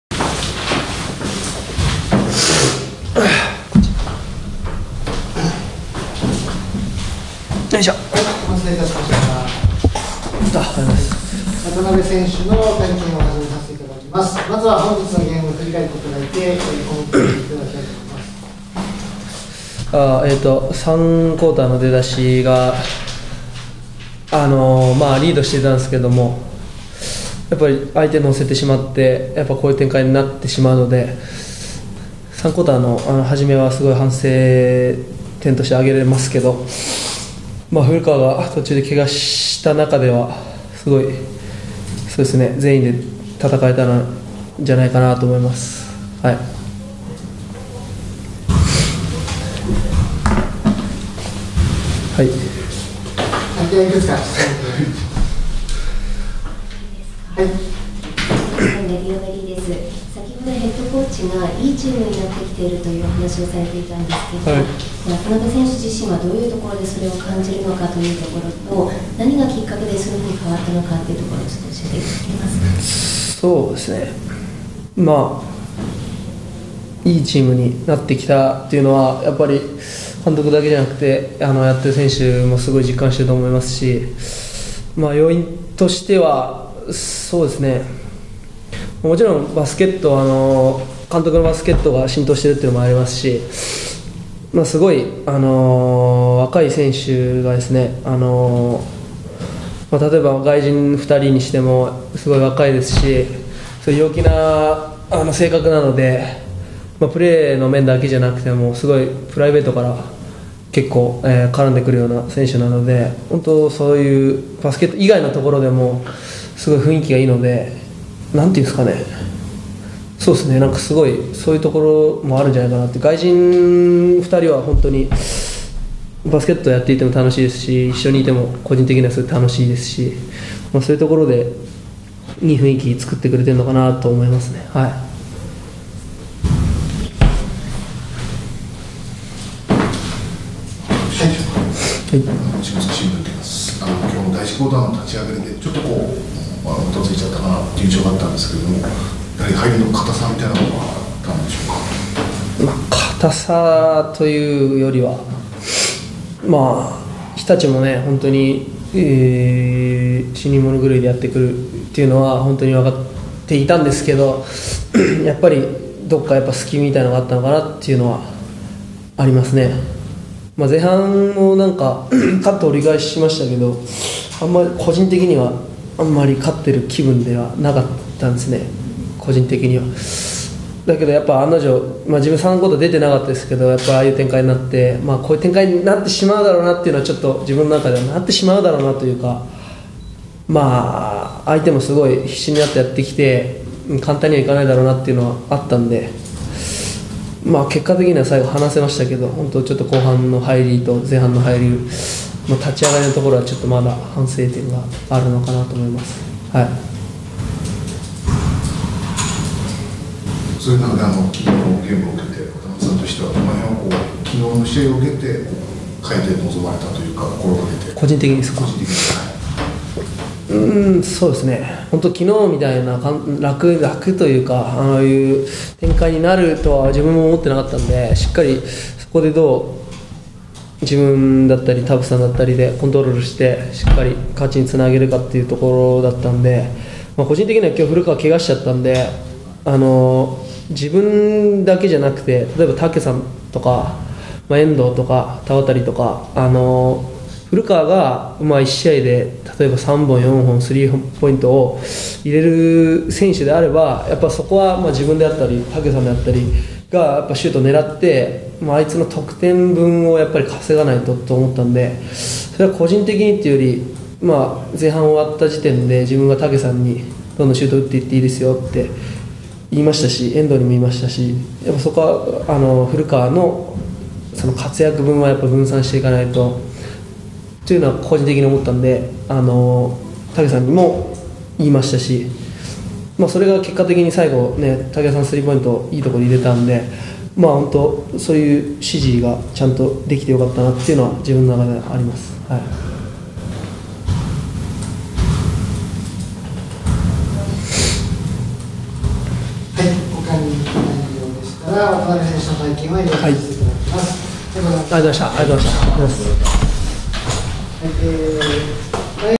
【記者会見】11/17（日）日立東京戦終了後の#13 渡邉選手の会見
NBL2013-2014レギュラーシーズンGame16・日立サンロッカーズ東京戦­終了後の#13 渡邉裕規選手の記者会­見です。